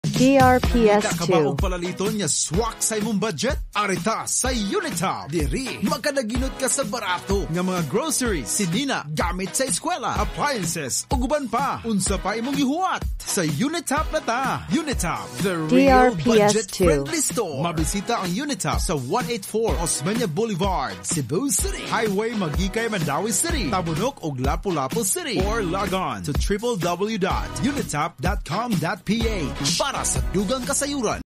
UNITOP Mall Cebu Radio Ad 2024 31s (Cebu)
Ito po ang 2024 Provincial Radio Ad ng UNITOP Mall Cebu.

DRPS Two 7223: UNITOP Mall Cebu Radio Ad 2024 31s (Cebu)